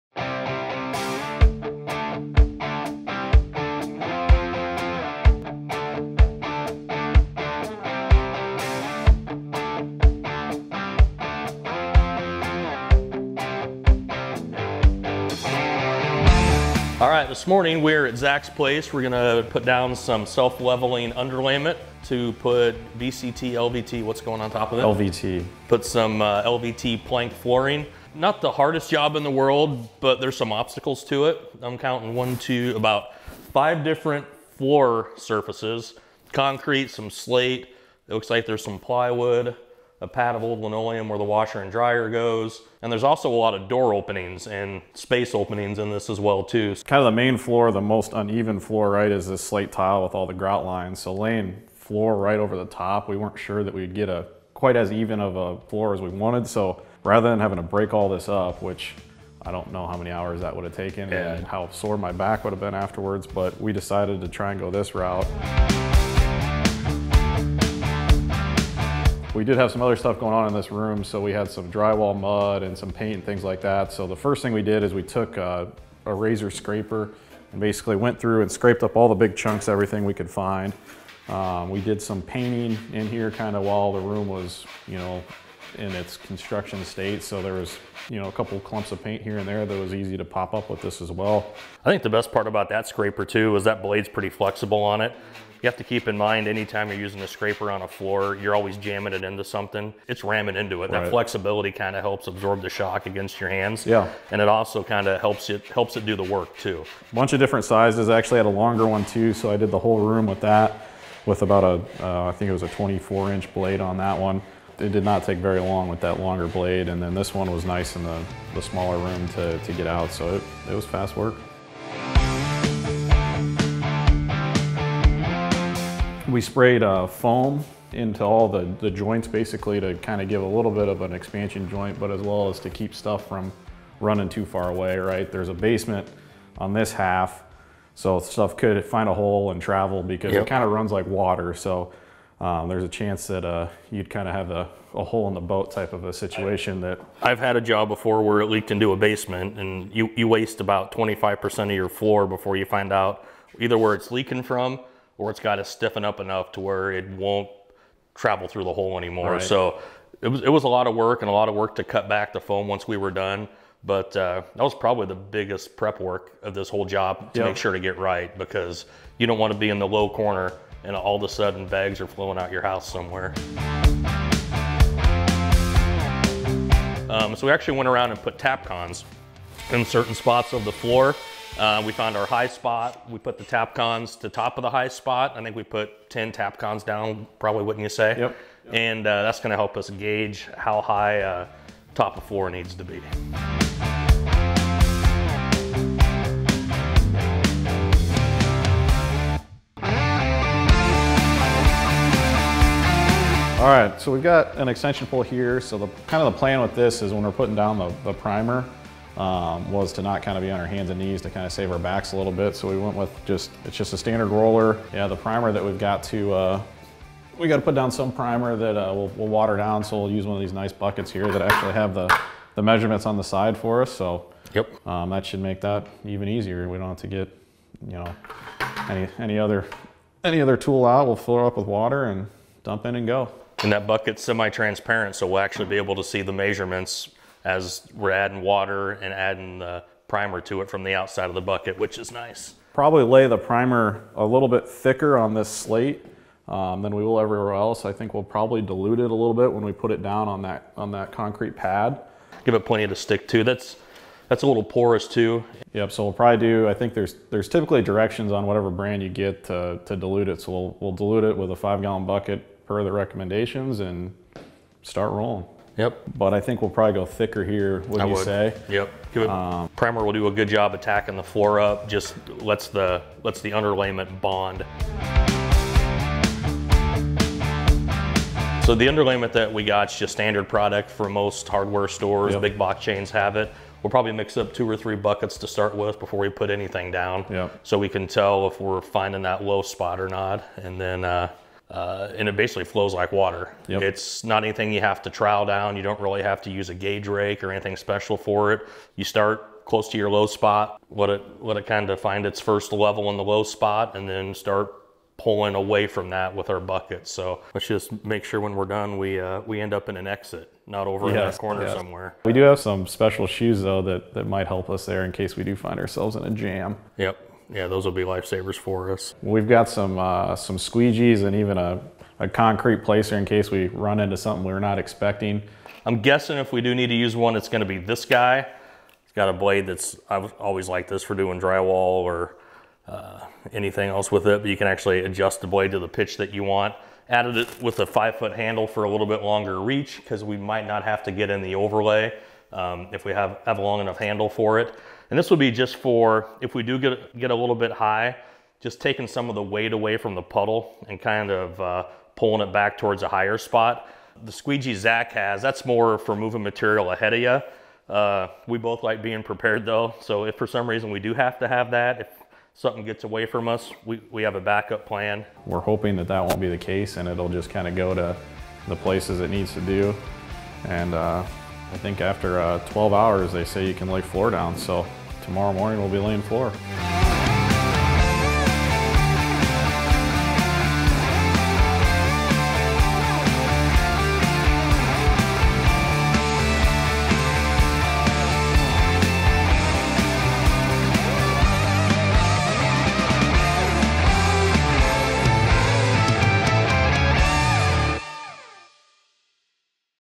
DIY Workshop Self Leveling Underlayment
Chapters:
00:00 Intro
01:07 Step 1 Clean up the floor surface
02:04 Step 2 Seal up cracks, holes, and voids in the floor
02:54 Step 3 Check the floor for level
03:21 Step 4 Apply primer to the floor
04:36 Step 5 Mix the underlayment